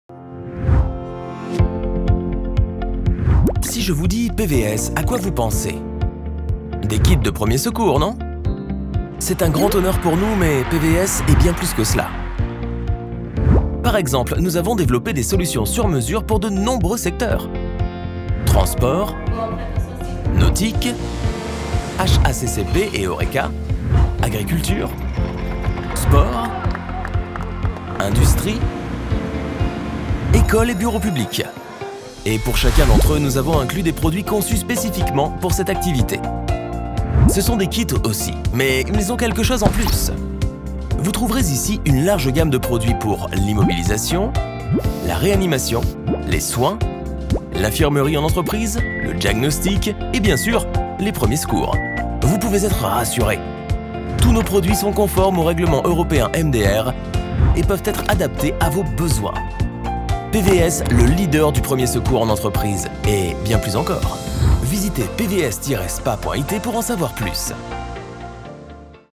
Gamme de produits PVS
PVS vous présente sa gamme complète de produits. Quand vous pensez à PVS, vous pensez sûrement à la trousse de premiers soins, mais PVS est beaucoup plus. Nous avons créé des solutions ad hoc pour de nombreux secteurs tels que la navigation de plaisance, l’agriculture, l’école, les bureaux, les transports, les sports, le haccp. Et en plus de nos kits, nous avons une large gamme de produits pour l’immobilisation, l’infirmerie d’entreprise, les premiers soins, le diagnostic, la réanimation, le pansement.
Pour en savoir plus, visitez notre site Web 👇🏻